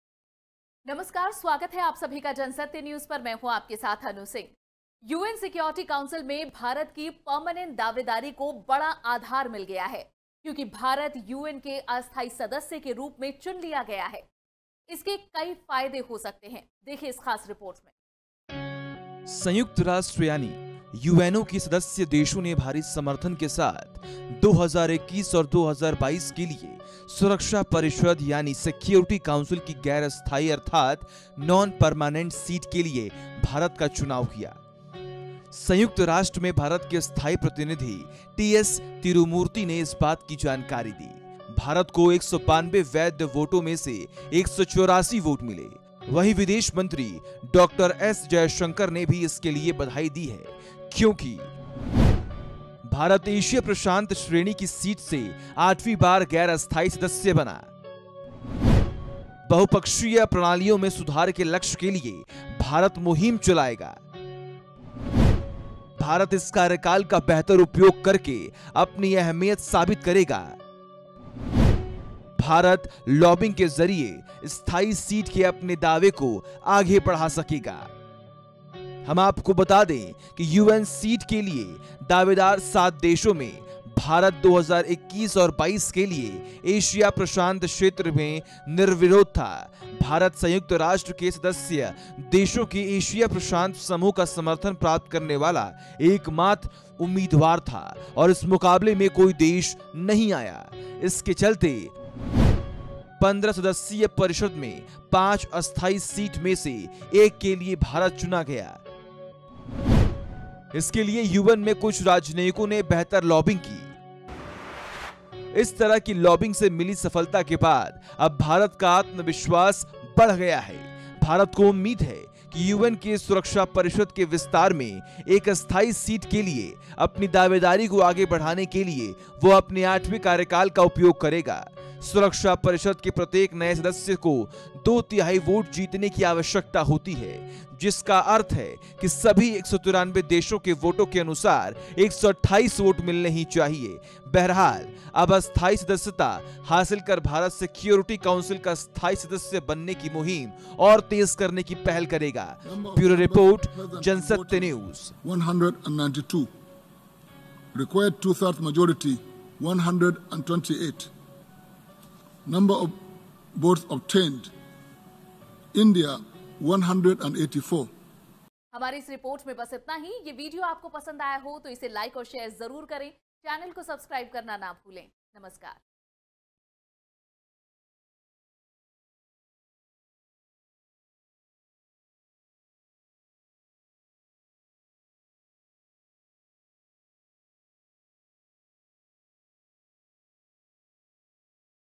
सिक्योरिटी काउंसिल में हिंदुस्तान की जय हो....
भारत को यूएन सिक्योरिटी काउंसिल में अस्थायी सदस्यता मिली है. जिसके कई फायदे हो सकते हैं देखिए एक रिपोर्ट..